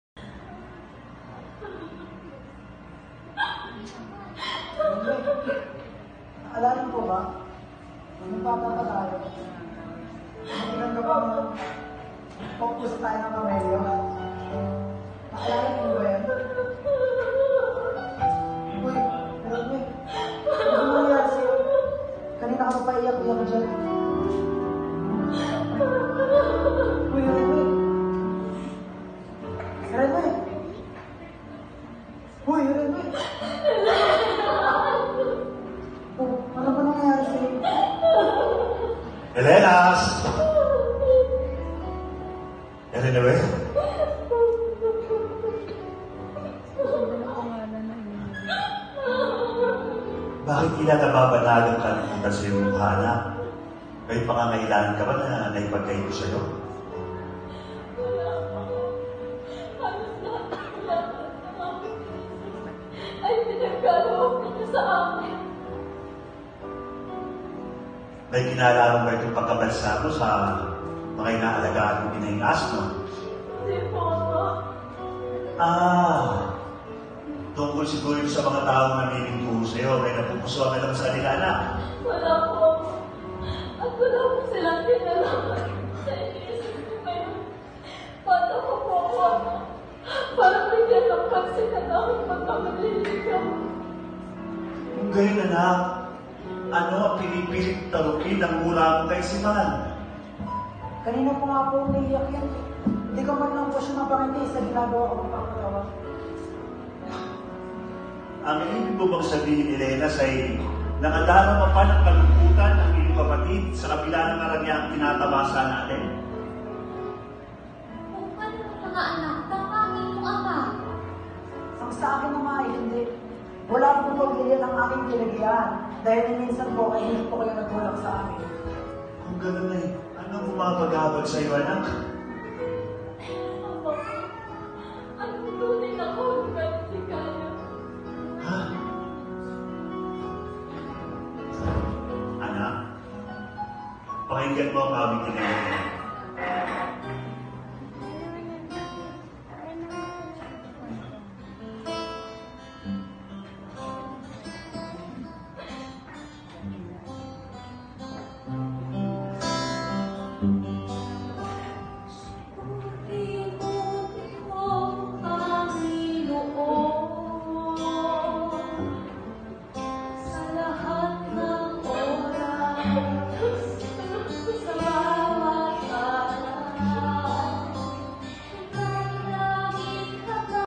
AGAM- AGAM NI JERENUE -a scene from "Ala ala ng Kahapon ni Job" directed by Direk John Novela
"Ala ala ng Kahapon ni Job"  is a theatrical show by Realm Productions and Realm Theatrical Center directed by Direk John Novela. A story based from a biblical character Job,  story of a man's faith to God.

This scene characters are:
Ivan James Delos Reyes as Jelenaz
Emirose Esmeña as Jerenue
Mj Bodigon Soriano as Elima
Krishnelle Balbada as Hana
Jordan Castillo as Job